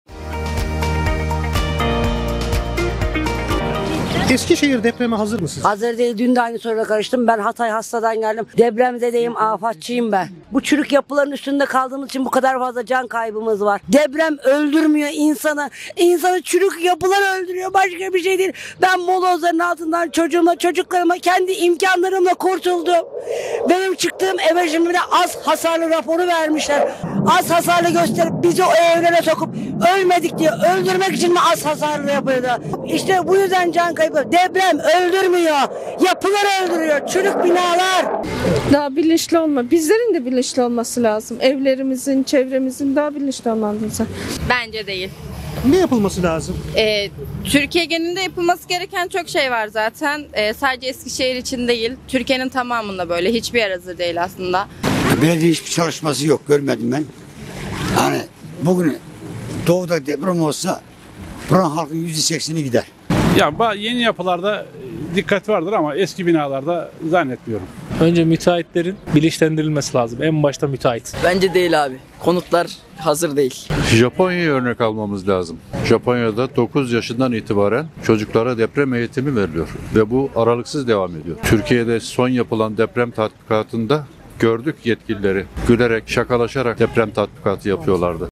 Eskişehir depreme hazır mı?
9 gün önce Kahramanmaraş merkezli ve 10 ilin etkilendiği şiddetli depremlerin ardından olası bir depremde 'Eskişehir ne kadar etkilenir?'' sorusunu akıllara getirdi.